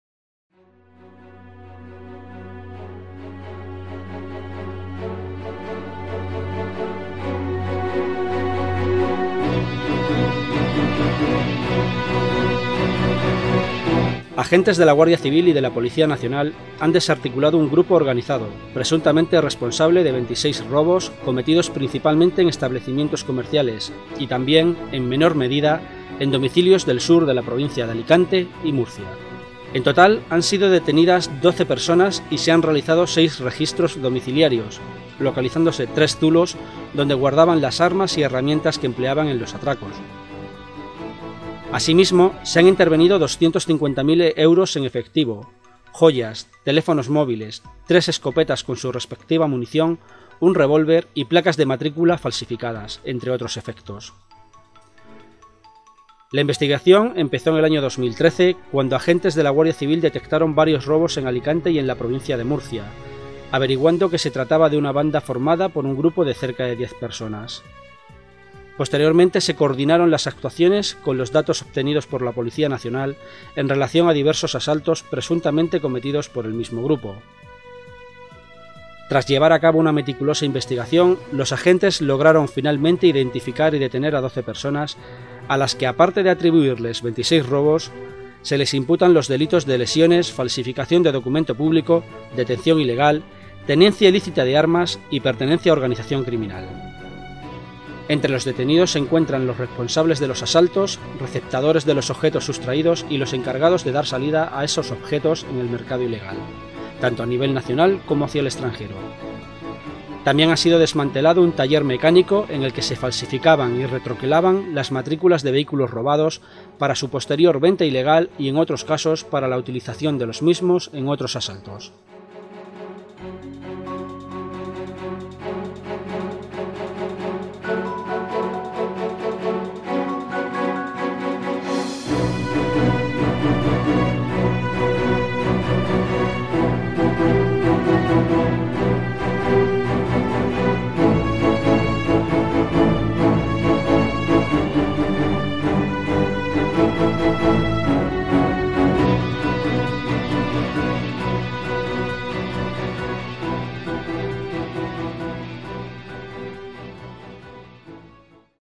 Operación Benemira. Operaciones de la Guardia Civil. Alicante. Conjunta con CNP
En una operación conjunta de la Guardia Civil y de la Policía Nacional

Desarticulado un grupo responsable de 26 robos en establecimientos comerciales y domicilios de Alicante y Murcia

Se han efectuado seis registros en los que se han intervenido 250.000 euros en efectivo, joyas y teléfonos, entre otros efectos, y se han localizado tres zulos donde escondían las armas y herramientas que utilizaban en los atracos

Actuaban de madrugada, ataviados con pasamontañas y guantes, y en la mayoría de los casos sustraían previamente un vehículo que utilizaban para cargar los efectos sustraídos y huir del lugar

11-febrero-2014.- Agentes de la Guardia Civil y de la Policía Nacional han desarticulado un grupo organizado presuntamente responsable de 26 robos cometidos principalmente en establecimientos comerciales y también, en menor medida, en domicilios del sur de la provincia de Alicante y del norte de la región de Murcia. En total han sido detenidas doce personas -en Orihuela, Elche, Carral, Cox, La Aljorra y Granja de Rocamora-, y se han realizado seis registros domiciliarios. Además se han hallado tres zulos donde guardaban las armas y herramientas que empleaban en los atracos y se han intervenido 250.000 euros en efectivo, joyas y teléfonos móviles, entre otros efectos.

La investigación empezó en el año 2013, cuando agentes de la Guardia Civil detectaron varios robos en Alicante -en la comarca de la Vega Baja y Crevillente- y en la provincia de Murcia. Tras las primeras pesquisas sobre la autoría de los atracos, averiguaron que se trataba de una banda formada por un grupo de cerca de 10 personas. Posteriormente se coordinaron las actuaciones con los datos obtenidos por la Policía Nacional en relación a diversos asaltos cometidos en su demarcación, presuntamente cometidos por el mismo grupo.
De madrugada y armados
El modus operandi era muy similar en todos los atracos que realizaron. Actuaban de madrugada, ataviados con pasamontañas y guantes, y en la mayoría de los casos sustraían previamente un vehículo que utilizaban para cargar los efectos sustraídos y huir del lugar de los hechos, abandonándolo una vez cometido el delito. Normalmente atracaban negocios de hostelería provistos de armas y otros objetos contundentes, con los que intimidaban a los trabajadores en el caso de que se encontrasen en el lugar en el momento del robo.
12 detenidos y 26 robos esclarecidos
Tras llevar a cabo una meticulosa investigación, los agentes lograron finalmente identificar y detener a 12 personas a las que se atribuyen 26 robos en viviendas y establecimientos de la Vega Baja alicantina. Asimismo se les imputan delitos de lesiones, falsificación de documento público, detención ilegal, tenencia ilícita de armas y pertenencia a organización criminal. Entre los detenidos se encuentran los responsables de los asaltos, los receptadores de los objetos sustraídos y los encargados de dar salida a esos objetos en el mercado ilegal, tanto a nivel nacional como hacia el extranjero. También han sido desmantelado, en la localidad alicantina de Cox, un taller mecánico en el que se falsificaban y retroquelaban las matrículas de vehículos robados para su posterior venta ilegal y en otros casos para la utilización de los mismos en otros asaltos.
En los seis registros domiciliarios que se efectuaron por los agentes en las localidades alicantinas de Orihuela (2), Elche (1), Catral (1) y Granja de Rocamora (2), se intervinieron 250.000 euros en efectivo, varias joyas, teléfonos móviles, prendas de vestir y herramientas utilizadas en los atracos y varios teléfonos y tarjetas sim, entre otros efectos. Además, se localizaron tres zulos -dos de ellos ubicados en campos de las afueras de Redován y Albatera (Alicante) y un tercero en Fuente Álamo (Murcia)-,  en los que se intervinieron 3 escopetas con su respectiva munición, un revólver, placas de matrícula falsificadas y otros útiles para los asaltos.
La investigación ha sido realizada por agentes de la  Unidad Orgánica de Policía Judicial de la Guardia Civil de Alicante y por la Brigada Provincial de Policía Judicial y de la Comisaría de Orihuela de la Policía Nacional.